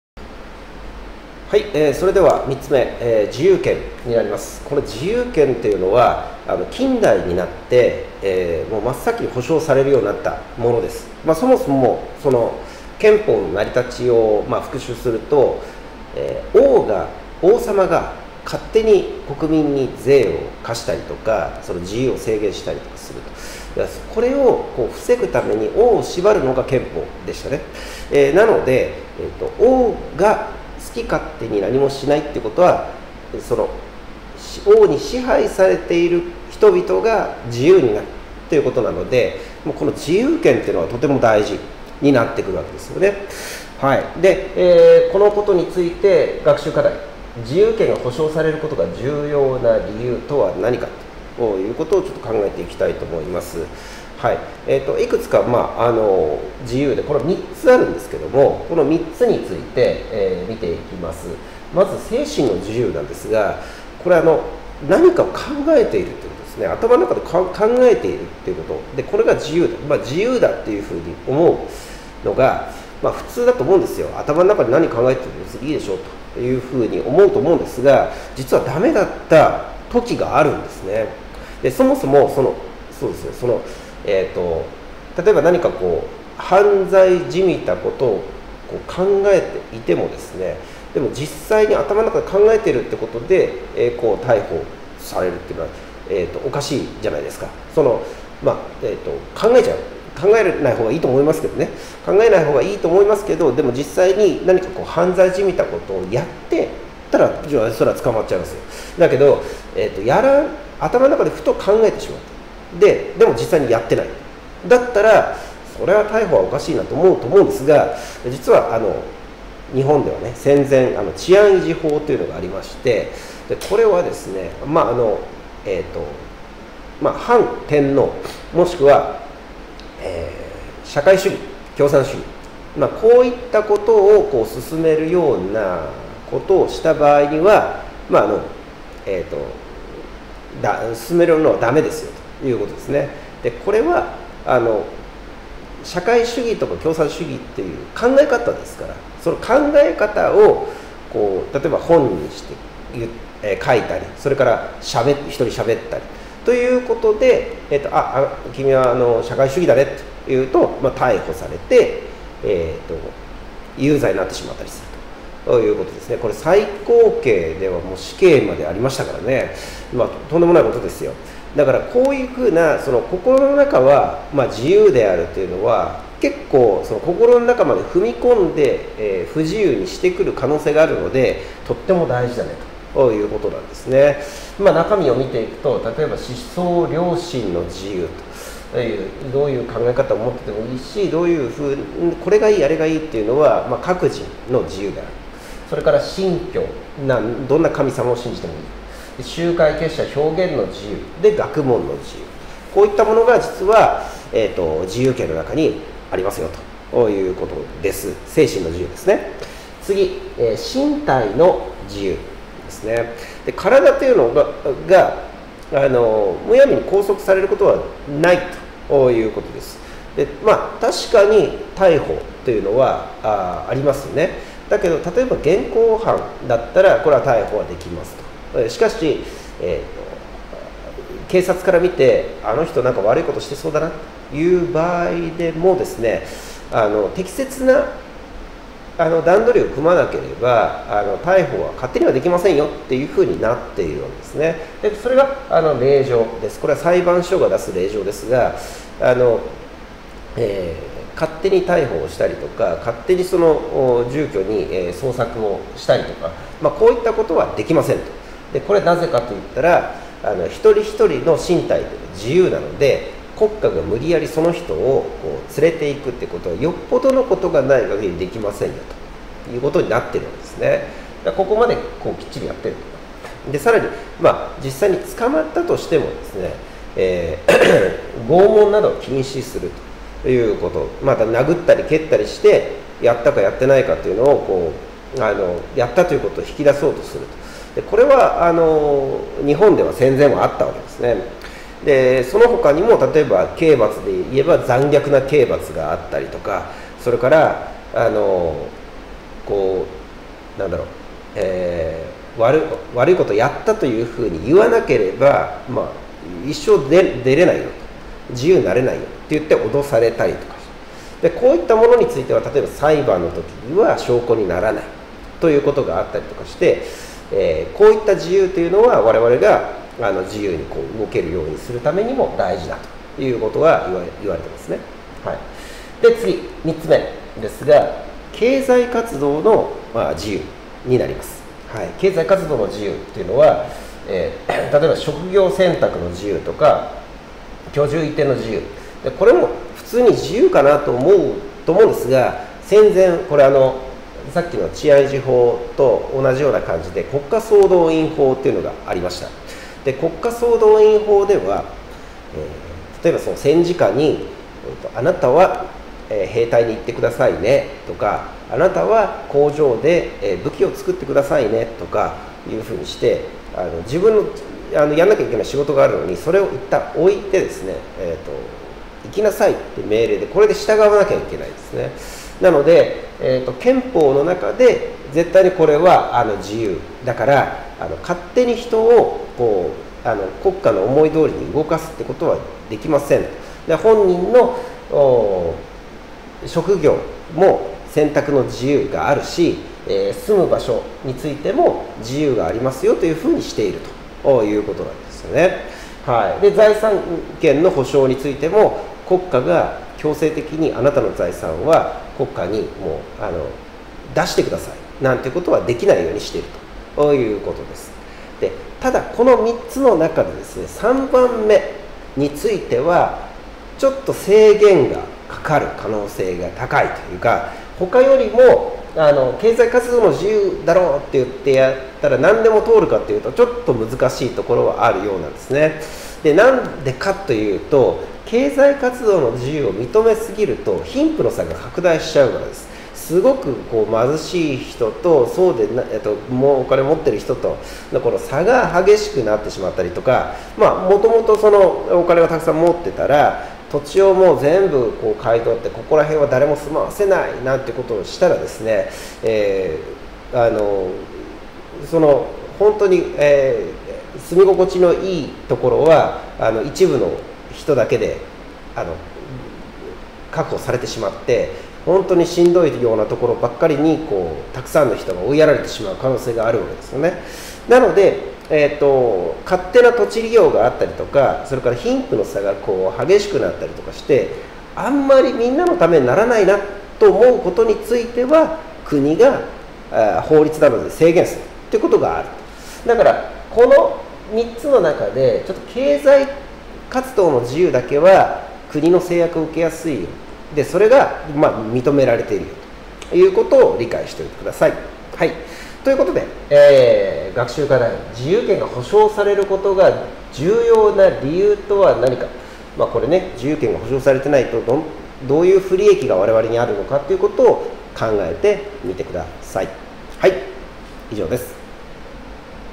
憲法⑧ 自由権(中学社会 公民的分野)
中学社会(公民)　教科書P54-55(東京書籍)
今回のテーマは「憲法⑧　自由権」です。